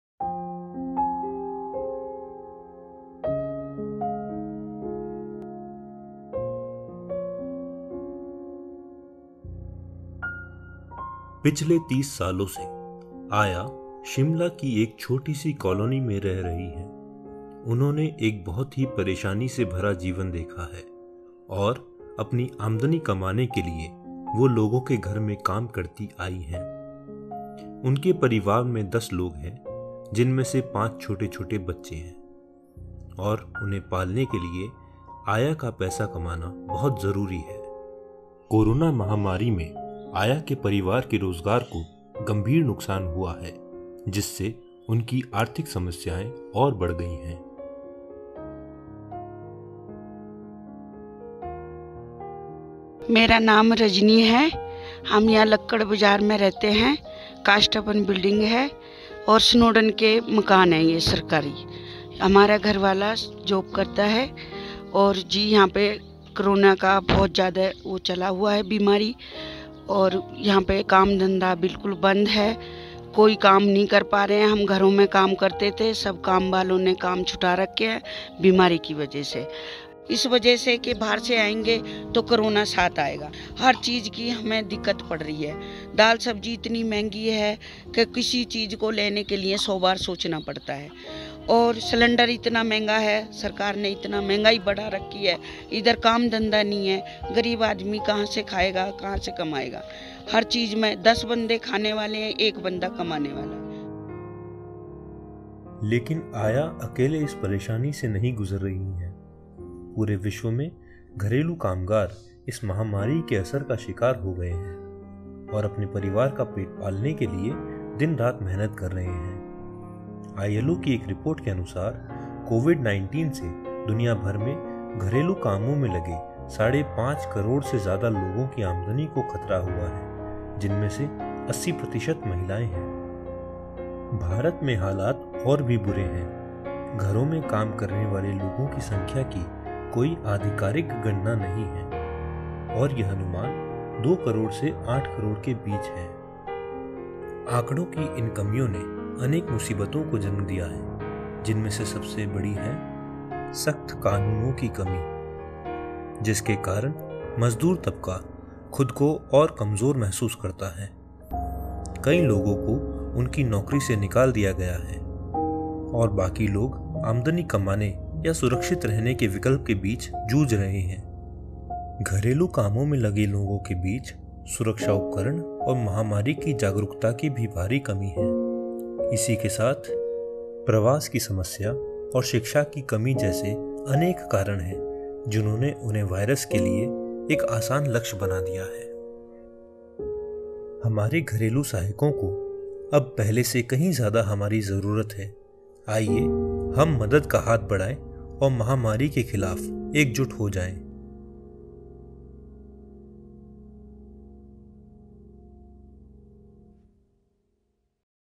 Aaya
The official data on domestic workers in India shows the involvement of at least 4.3 million workers in the sector, a number which according to the latest ILO report is a severe underrepresentation, with the estimated statistics standing at somewhere between 18 to 20 million. These workers are mostly illiterate, unorganized, over exploited and underpaid which makes them extremely vulnerable.

They lack social security and employment benefits, a situation that has just gotten worse with the onset of the Covid-19 pandemic. The video aims to shed light on this issue by following the case study of Rajni, popularly known as “Aaya” to build a personal connect with the audience.